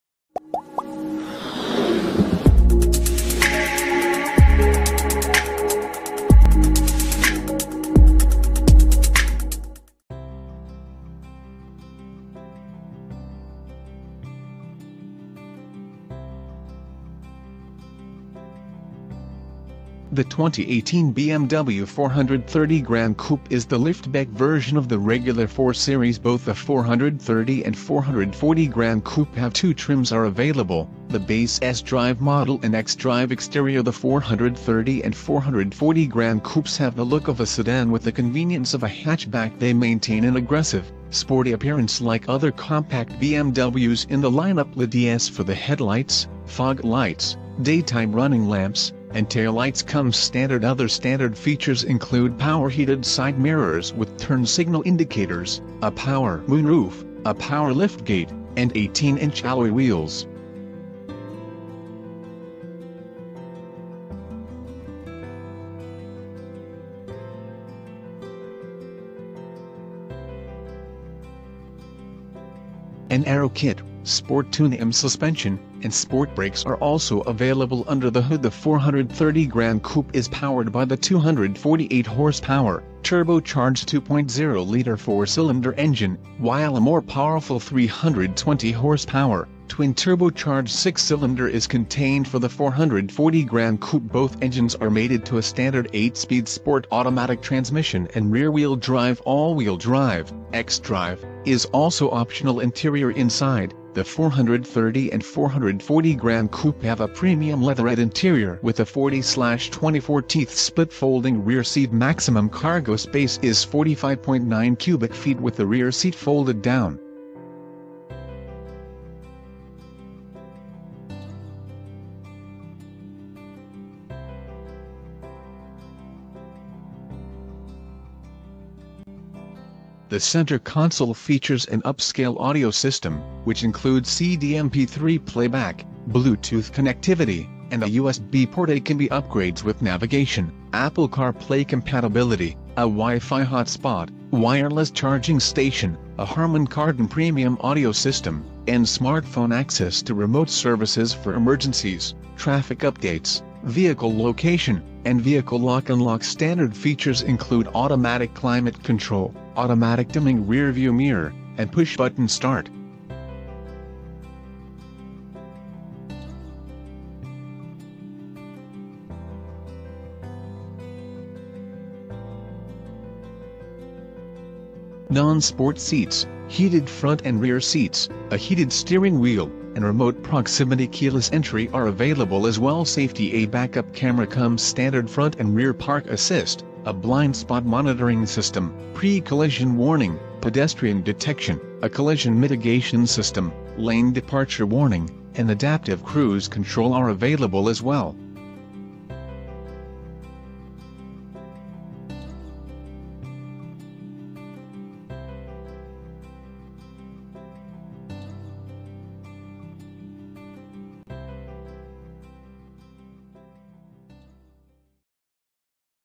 2018 BMW 430 Gran Coupe: REVIEW
The 2018 BMW 430 Gran Coupe is the liftback version of the regular 4 Series.

Both the 430 and 440 Gran Coupe have two trims are available: the base sDrive model and xDrive.

Exterior
The 430 and 440 Gran Coupes have the look of a sedan with the convenience of a hatchback. They maintain an aggressive, sporty appearance like other compact BMWs in the lineup. LEDs for the headlights, fog lights, daytime running lamps, and taillights come standard. Other standard features include power/heated side mirrors with turn signal indicators, a power moonroof, a power liftgate, and 18-inch alloy wheels. An aero kit, sport-tuned M suspension, sport brakes are also available.

The 430 Gran Coupe is powered by the 248-hp, turbocharged 2.0-liter 4-cylinder engine, while a more powerful 320-hp, twin-turbocharged 6-cylinder is contained for the 440 Gran Coupe. Both engines are mated to a standard 8-speed sport automatic transmission and rear-wheel drive. All-wheel drive (xDrive) is also optional.

Interior
Inside, the 430 and 440 Gran Coupe have a premium leatherette interior with a 40/20/40-split folding rear seat. Maximum cargo space is 45.9 cubic feet with the rear seat folded down. The center console features an upscale audio system, which includes CD/MP3 playback, Bluetooth connectivity, and a USB port. It can be upgrades with navigation, Apple CarPlay compatibility, a Wi-Fi hotspot, wireless charging station, a Harmon Kardon premium audio system, and smartphone access to remote services for emergencies, traffic updates, vehicle location, and vehicle lock/unlock. Standard features include automatic climate control, automatic dimming rearview mirror, and push-button start. Non-sport seats, heated front and rear seats, a heated steering wheel, and remote proximity keyless entry are available as well.

Safety
A backup camera comes standard. Front and rear park assist, a blindspot monitoring system, pre-collision warning, pedestrian detection, a collision mitigation system, lane departure warning, and adaptive cruise control are available as well.